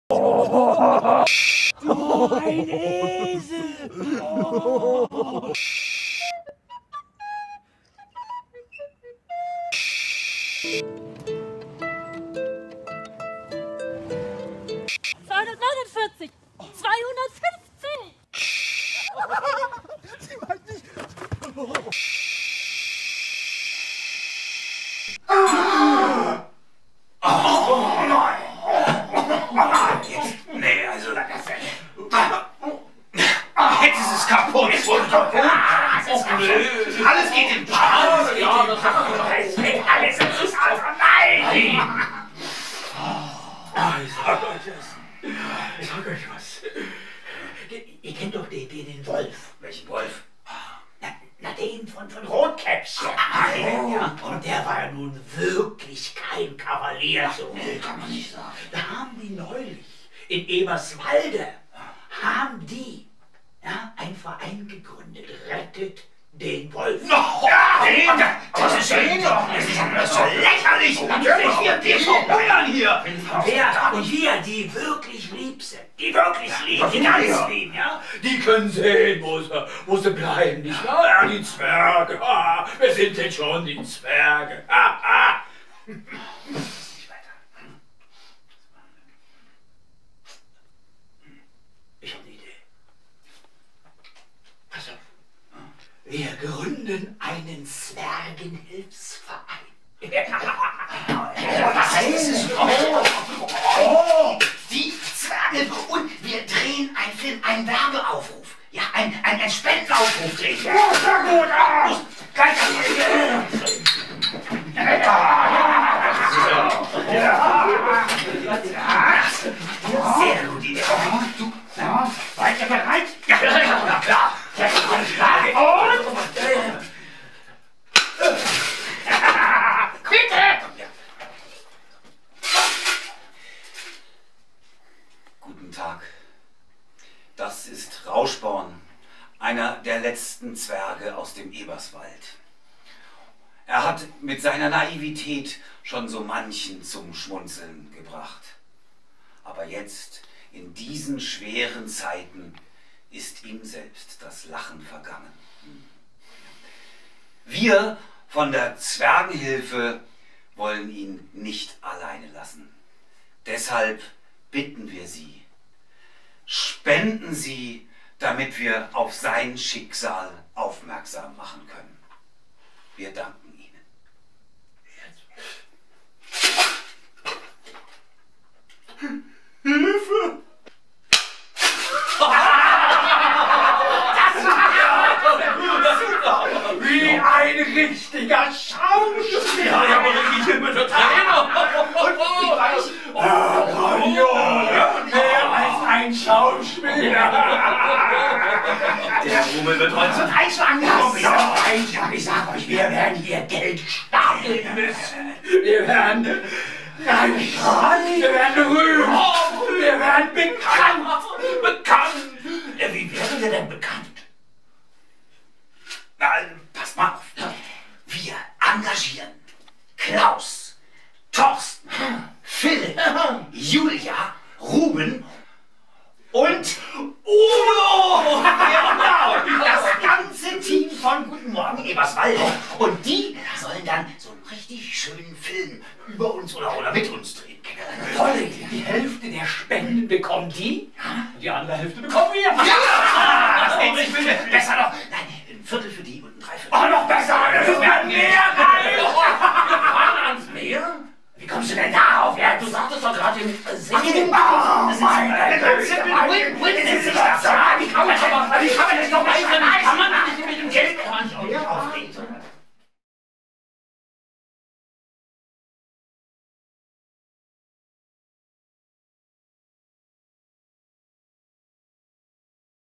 Spendenaufruf Zwergenhilfe: Für neue Folgen aus dem Eberswald!
Am 20. und 27. März 2021 möchten wir die Geschichte "Guten Morgen aus dem Eberswald" fortsetzen. Für die Produktion und Live-Sendung einer Folge benötigen wir 3.333 € - zwei Folgen kosten 6.666 €. Beteilige dich jetzt am Crowdfunding für dieses Projekt:

Alle drei bisherigen Folgen der Reihe könnt ihr hier auf unserem YouTube-Kanal ansehen:

Zwergenhilfe
Produktion: Torsten Stapel, Udo Muszynski, Julia Heilmann
Bildschnitt, Kamera, Ton, Licht: Torsten Stapel
Drehbuch und Spiel: Aktionstheater Kamaduka